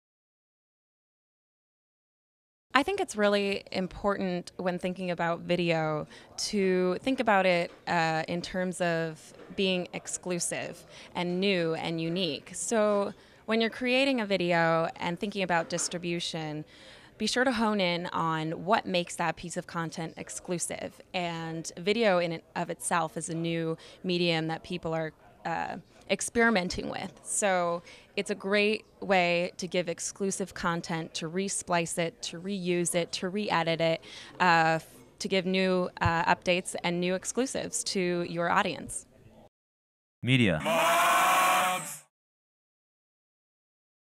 Business Wire L.A. Event With Meredith Kendall
Merideth Kendall of Break Media speaks out on video exclusivity